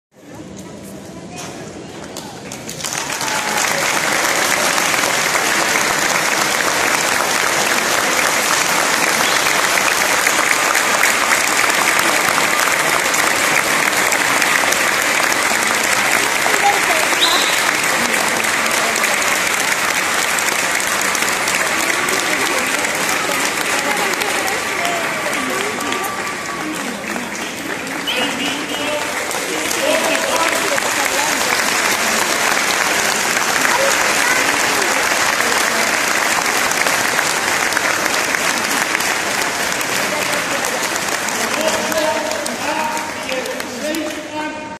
Coro oficial hace su ingreso y felices con aplausos del publico
Sinfonía por el Perú coro oficial ingreso